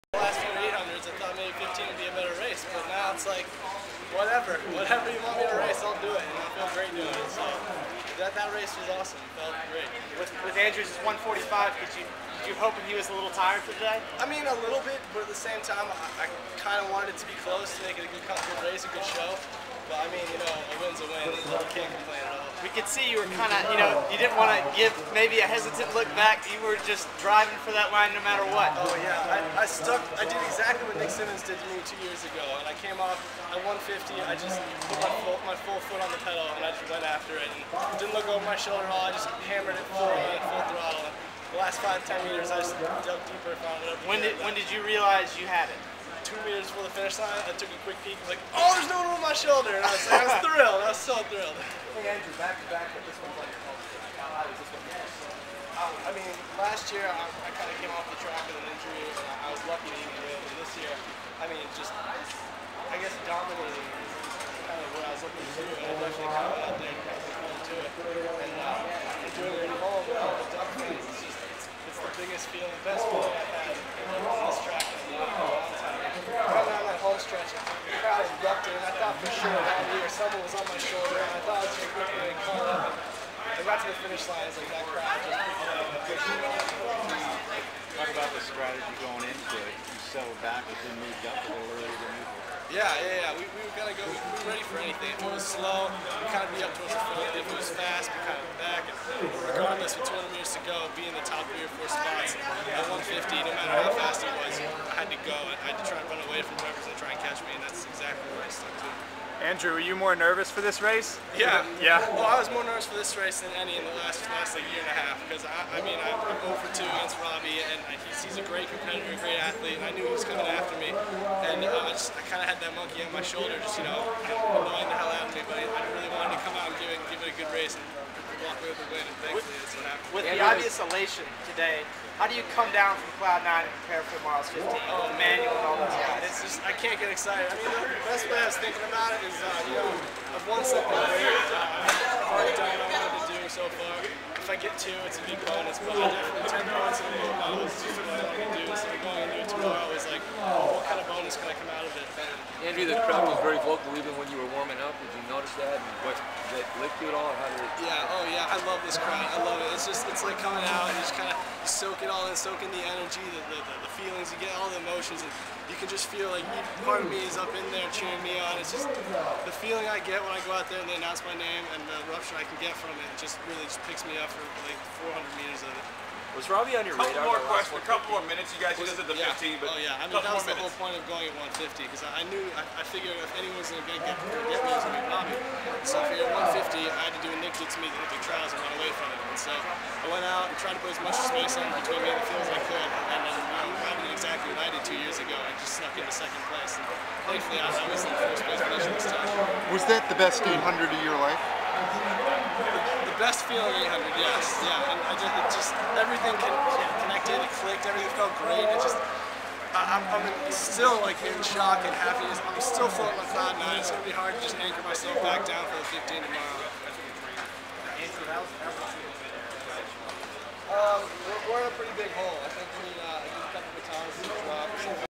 Wheating after winning 2010 NCAA 800
Andrew Wheating talks to reporters after getting a huge monkey off his back and winning the NCAA 800 title over Robby Andrews.